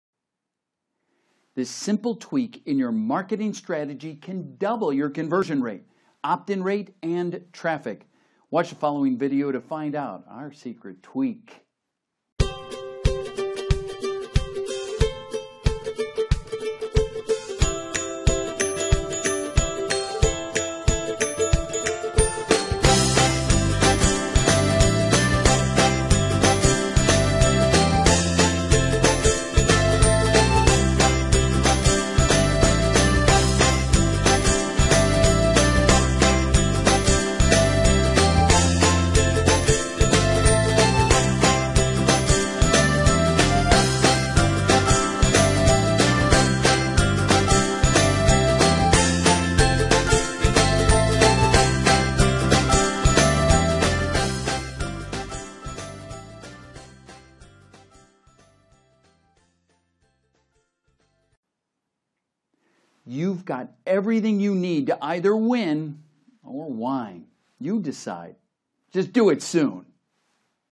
How To Get Leads and Follow Up With Them
Mobile Marketing is here to stay. Why? All your potential customers have a Mobile Phone and you now have the software to contact them as needed!